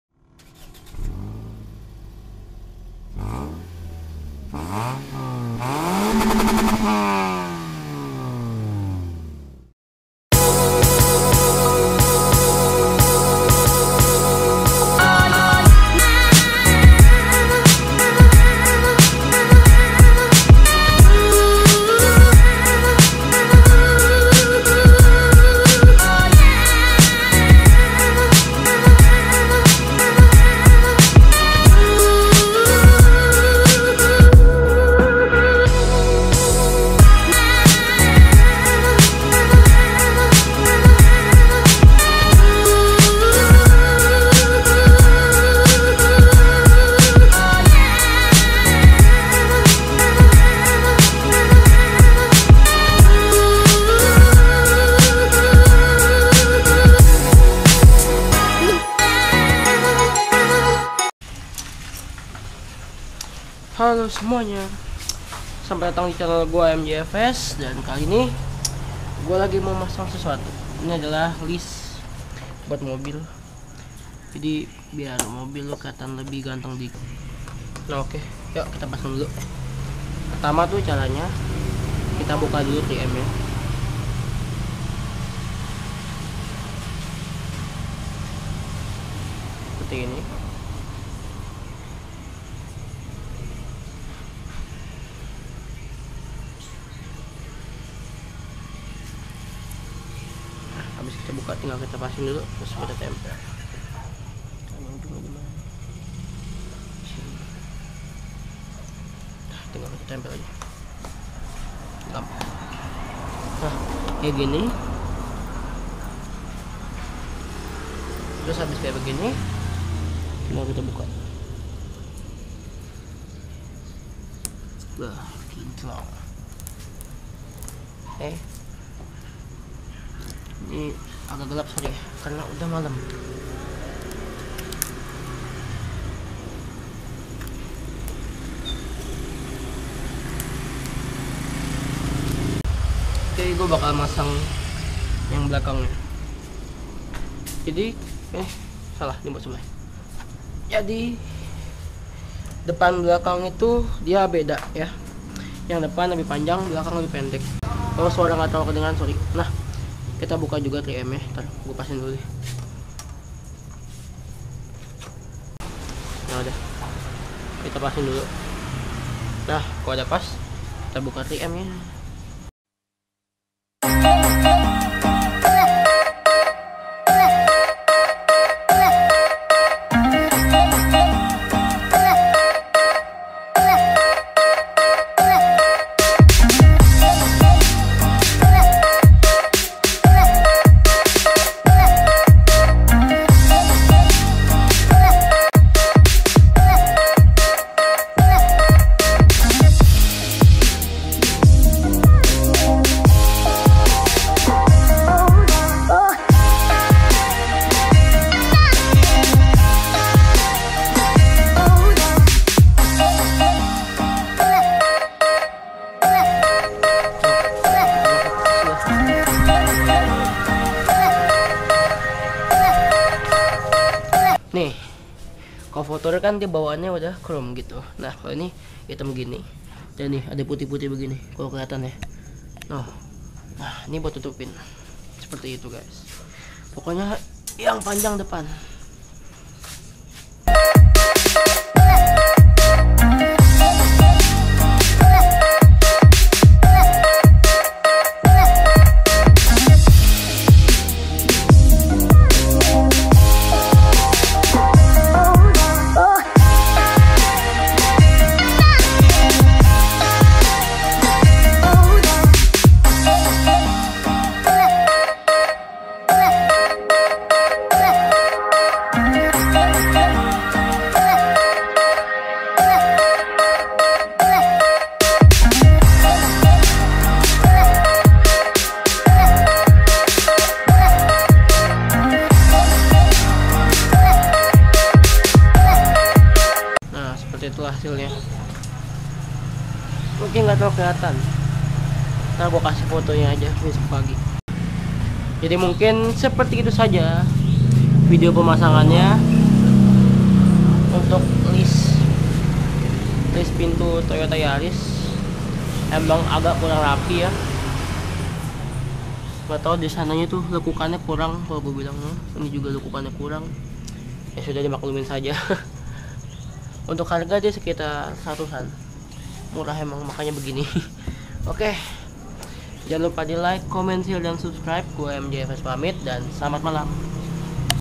Pasang List Chrome Di New Yaris
Halo Guys Sorry banget Lagi Jarang Upload, Dan Kali Ini gw mau mempercantik sedikit mobil gw dengan menambahkan list chrome, mau tau gimana pasangnya dan hasilnya?, mari di tonton haha